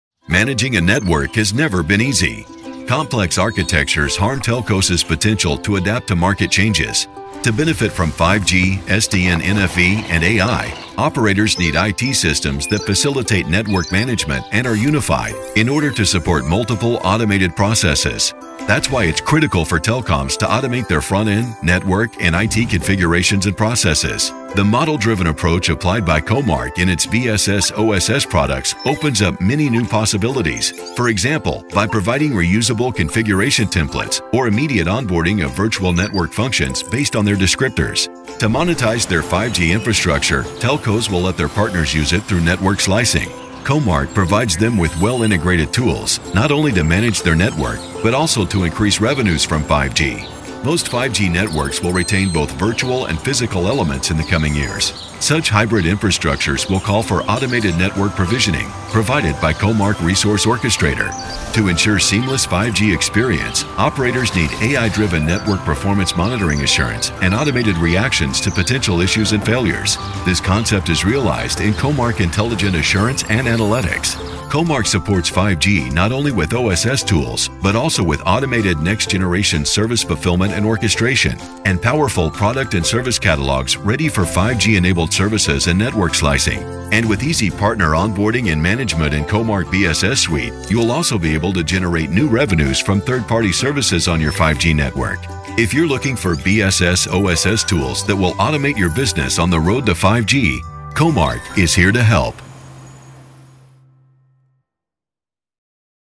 5G Network Automation Opportunities and Challenges - COMARCH
While 5G and automation go hand-in-hand, telco operators need an intelligent approach to network development in order to make the most of the opportunities on offer. Our new episode explains how telcos can reap the rewards of 5G by adopting a sharply focused approach to automation in order to enhance customer experience and meet business goals.